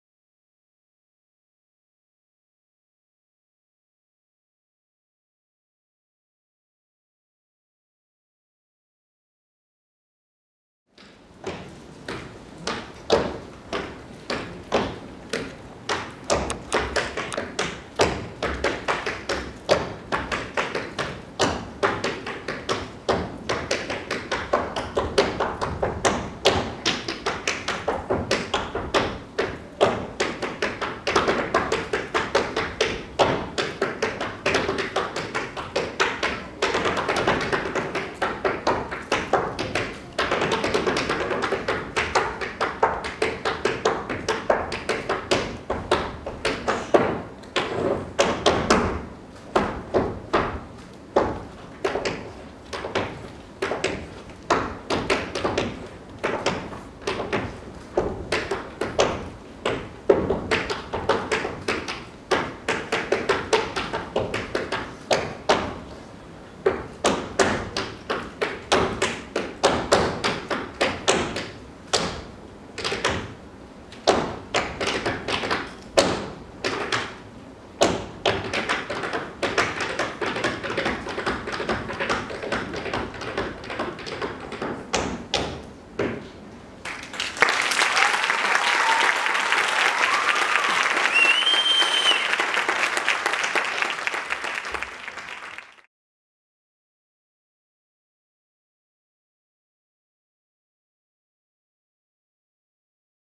À Capella - Festival de Outono 2018
À Capella: Tamires Coutinho

Festival de Outono 2018 - Gisele Alvim Espaço de Dança

Teatro Fashion Mall - Dia 16 de Junho